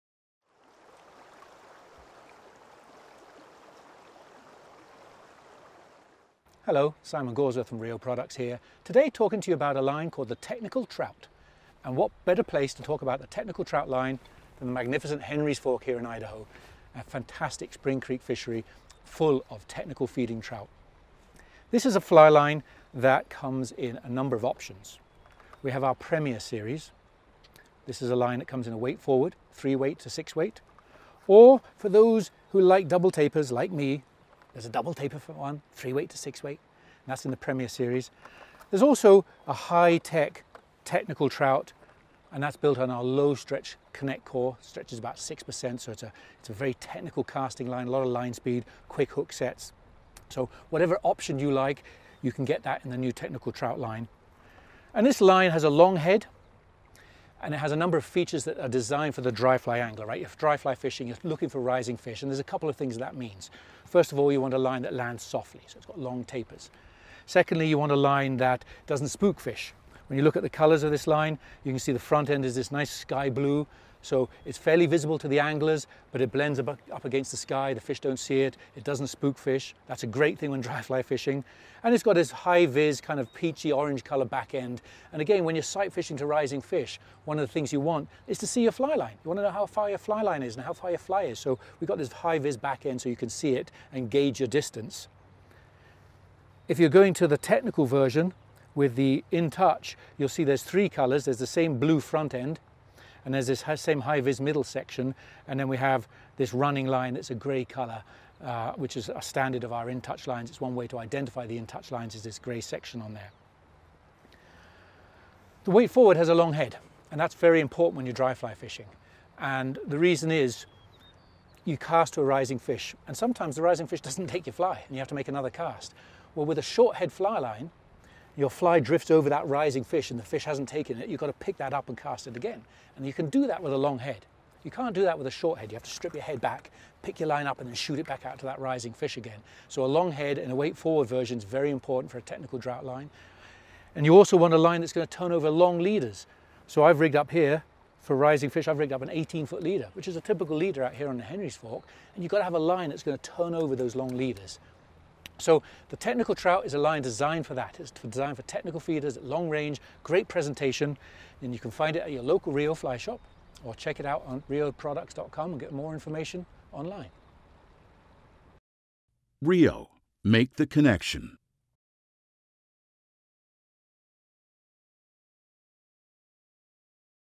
RIO's Technical Trout Fly Line
This short film introduces RIO's new Technical Trout fly line, and explains the highlights and benefits that this line design has for the trout angler.

RIO's Technical Trout line line features a long, fine front taper for the lightest of presentations and delicacy and is the ideal choice of line when fishing dry flies, nymphs, soft hackles and emergers to tricky eaters at distance. The line has a long head and back taper to increase loop control when carrying long lengths, and for making precise casts to rising fish, while the weight distribution makes it easy to turn over long leaders for technical feeders at range.

The line is available in an ultra-low stretch ConnectCore option, that  provides incredible levels of sensitivity when casting and fishing, as well as RIO's regular Premier series with approximately 30% stretch.